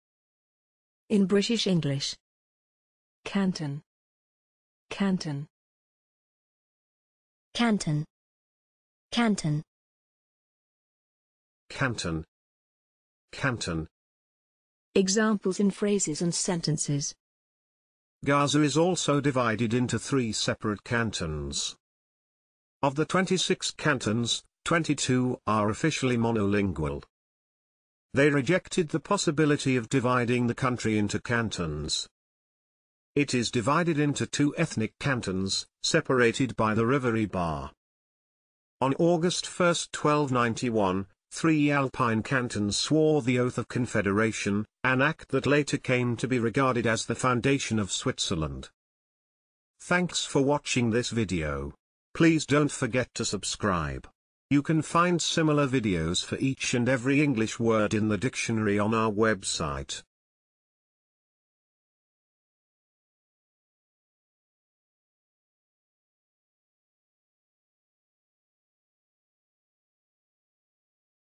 canton - pronunciation in British English (three voices / accents)
This video shows the pronunciation of canton in British English by three different voices/accents.